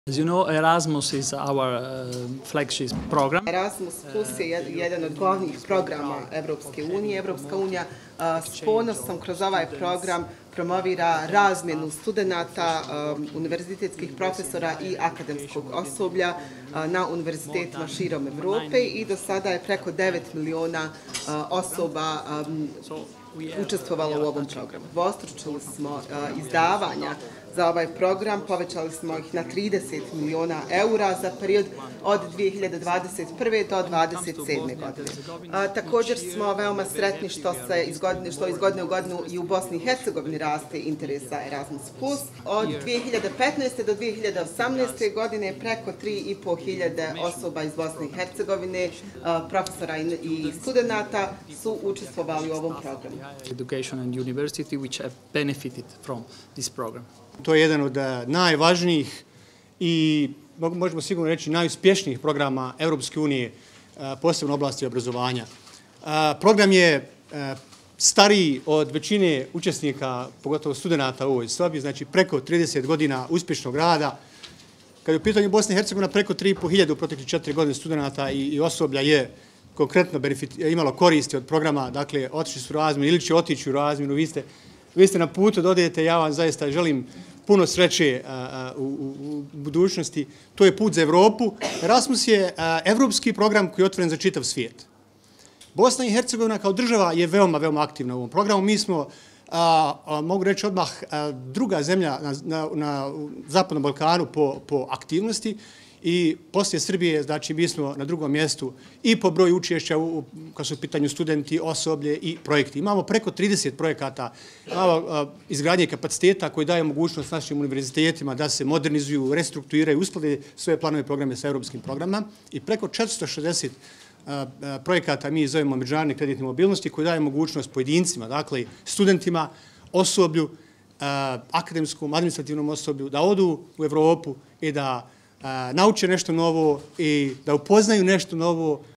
Udvostručen budžet Erasmus+ programa, sve veći interes u BiH
SARAJEVO, 25. juna (FENA) - Za Erasmus+ programe koji će biti implementirani od 2021. do 2027. godine Evropska komisija udvostučila je budžet, na 30 milijardi eura, što će omogućiti da u programu učestvuje 12 miliona ljudi.